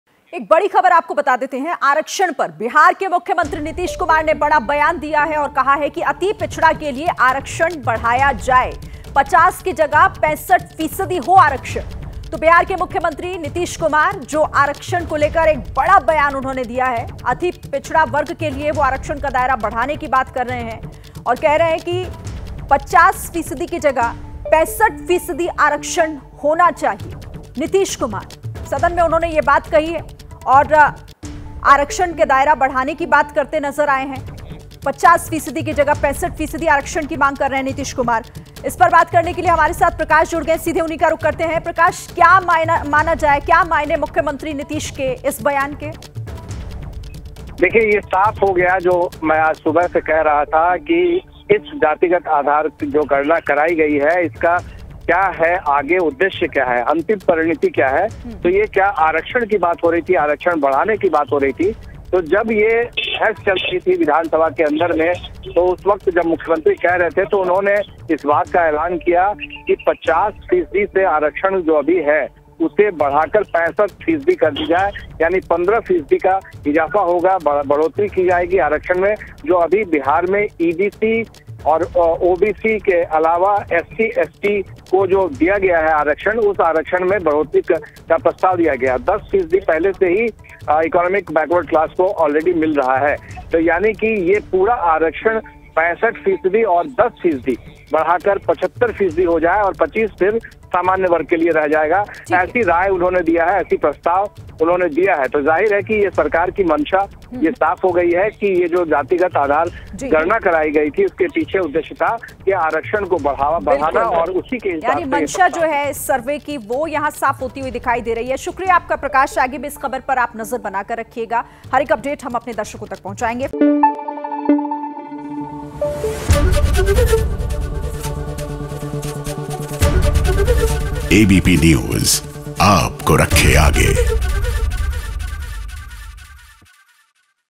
Nitish Kumar On OBC Reservation : 'अति पिछड़ा के लिए आरक्षण बढ़ाया जाए'- नीतीश कुमार | Bihar news
बिहार सरकार ने मंगलवार (07 नवंबर) को विधानसभा के शीतकालीन सत्र (Bihar Vidhan Sabha Winter Session) में जातीय गणना (Caste Survey) से जुड़ी रिपोर्ट पेश की. मुख्यमंत्री नीतीश कुमार (Nitish Kumar) ने सदन में आरक्षण बढ़ाने के मुद्दे पर बड़ा प्रस्ताव रखा है. नीतीश कुमार ने कहा कि पिछड़े और अतिपिछड़े का आरक्षण बढ़ना चाहिए. 50 की जगह 65 फीसद किया जाना चाहिए. पहले से अगड़ी जातियों को 10 प्रतिशत है तो इस 65 प्रतिशत के बाद कुल आरक्षण 75 प्रतिशत हो जाएगा, तब अनारक्षित 25 प्रतिशत बचेगा.

नीतीश कुमार ने एससी के लिए 20, एसटी के लिए 2 और ओबीसी एवं ईबीसी के लिए 43 फीसद आरक्षण देने का प्रस्ताव रखा. बता दें कि बिहार में ईबीसी के लिए आरक्षण 10 फीसद, एससी के लिए 16, एसटी के लिए 01, ईबीसी के लिए 18, ओबीसी के लिए 12 और ईबीसी एवं ओबीसी महिलाओं के लिए तीन फीसद है.